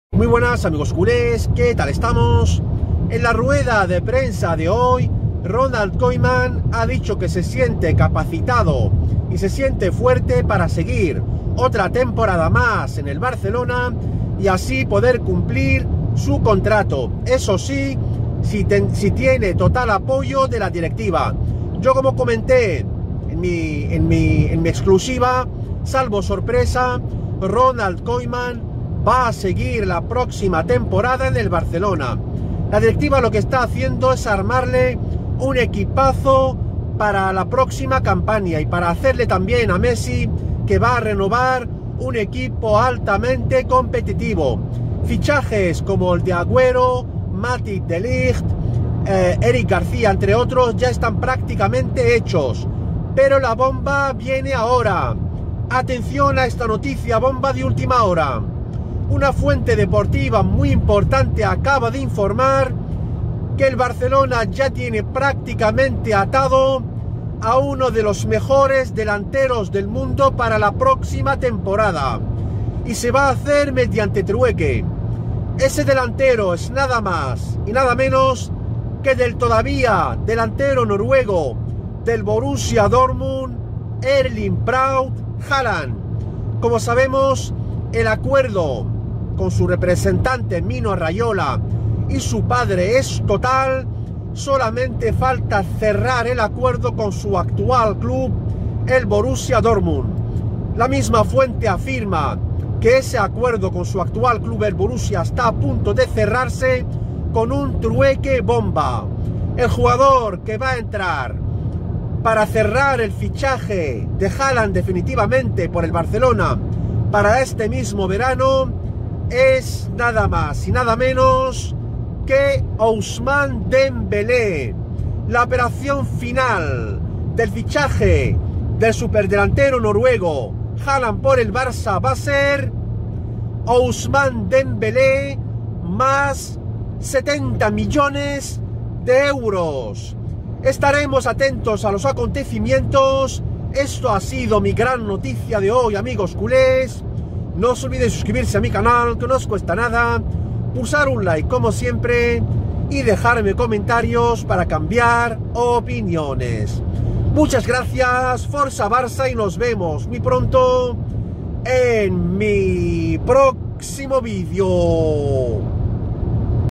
ÚLTIMA HORA ¡¡MESSI SE EMOCIONA CON ESTE CRACK MUNDIAL QUE FICHA EL BARCELONA CON TRUEQUE BOMBA!!
Messi se emociona con el crack mundial que acaba de fichar el Barcelona